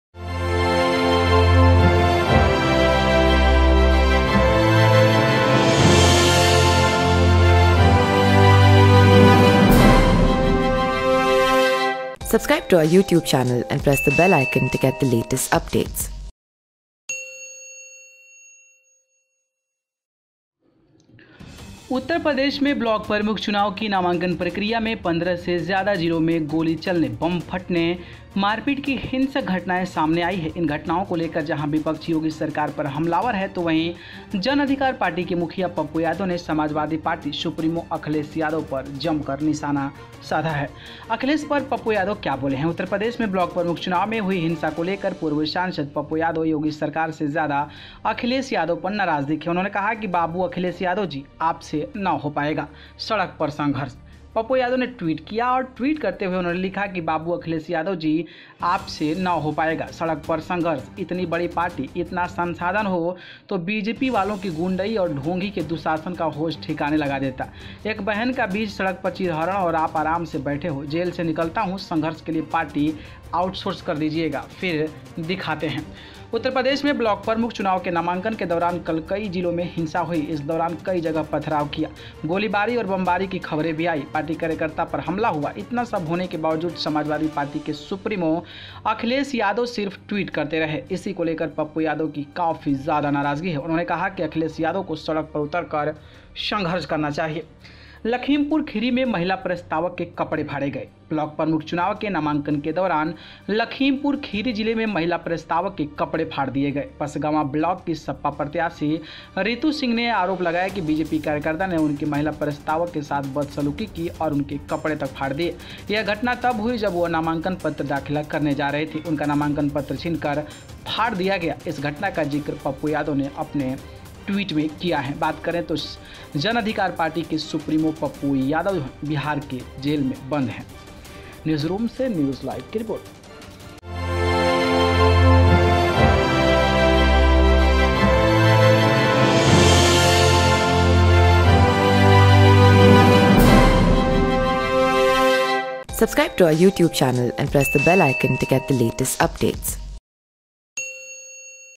यूपी में चुनावी हिंसा पर Pappu Yadav ने Akhilesh Yadav को लताड़े बोले - बाबू अखिलेश आप से न हो पाएगा'
यूपी में चुनावी हिंसा पर Pappu Yadav ने पूर्व CM Akhilesh Yadav को लताड़े बोले - बाबू अखिलेश यादव जी, आप से न हो पाएगा'